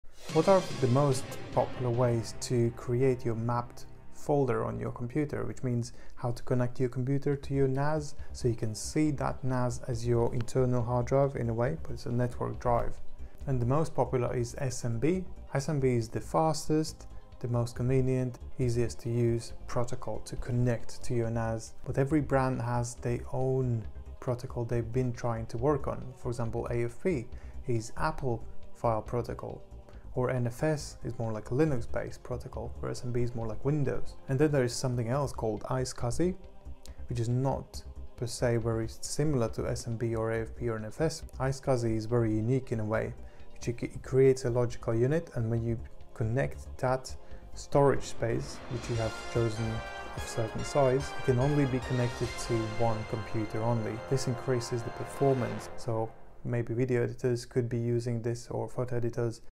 Understanding SMB and Other Network Protocols: Optimize Your NAS Connectivity
Explore the most popular network protocols for connecting your computer to a NAS, transforming it into an efficient network drive. This video highlights SMB (Server Message Block), the fastest and easiest protocol widely used for Windows environments. Additionally, learn about AFP (Apple Filing Protocol) for Apple devices, NFS (Network File System) for Linux systems, and iSCSI, which is unique for creating dedicated storage volumes for high-performance needs like video editing. Discover which protocol best suits your setup for optimal performance and convenience in managing networked storage solutions.